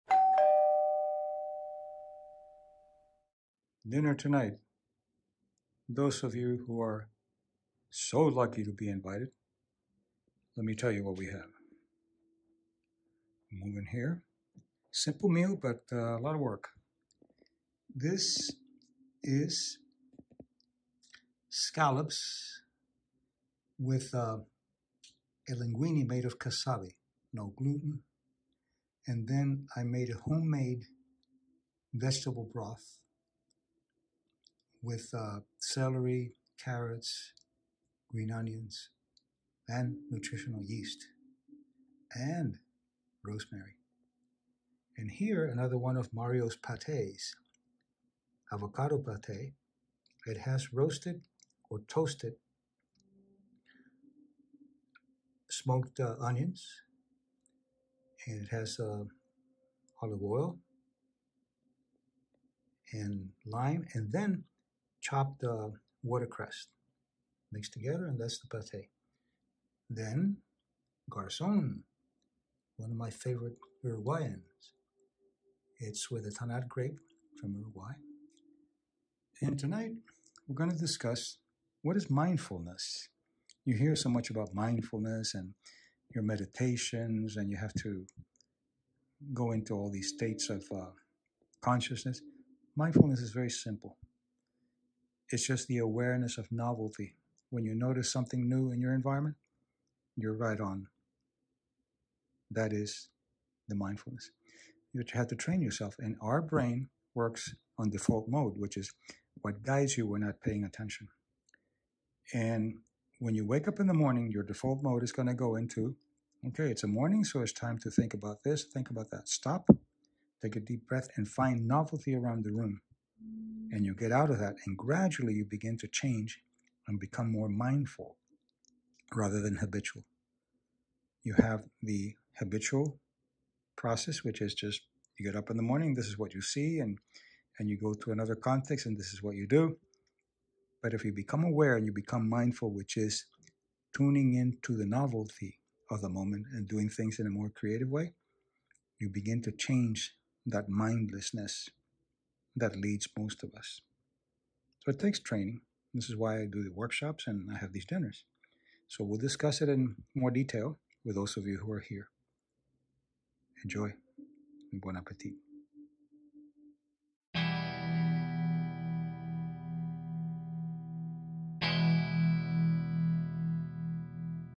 Demystifying Mindfulness
Learning what mindfulness really is, how to access it, and how to benefit from novelty learning.